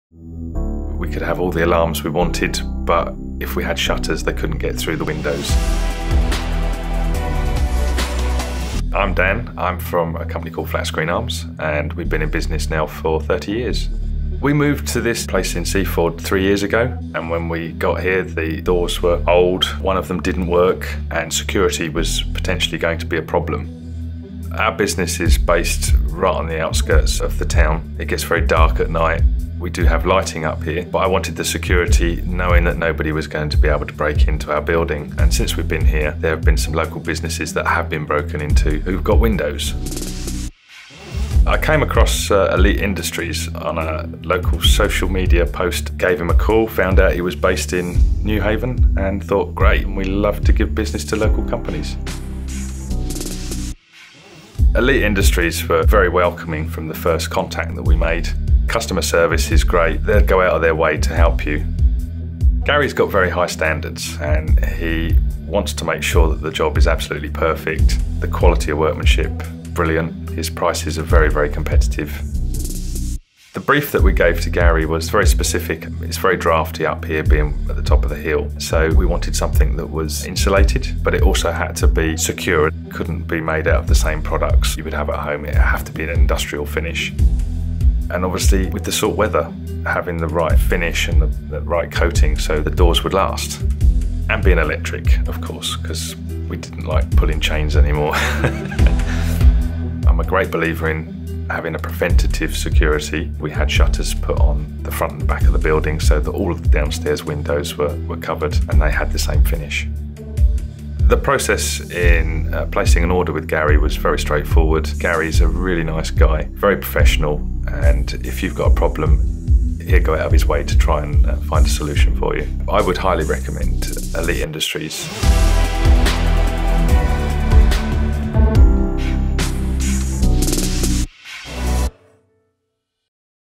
Industrial shutter door testimonial for Elite Industries of Newhaven
Flatscreens of Seaford describe the process of selecting the right company to manufacture and install insulated industrial shutter doors for their company headquarters in Seaford.

The company selected Elite Industries for the following reasons: the industrial roller shutter doors, and window shutters, had to be secure, insulated and finished with a coating that protected them from the windy, and often salty coastal weather.

Gary Walsh, Director of Elite Industries came up with exactly the right solution and the video shows the quality of the project and how the doors look today in their industrial setting.

To find out more about Elite Industries Ltd. visit their or call them on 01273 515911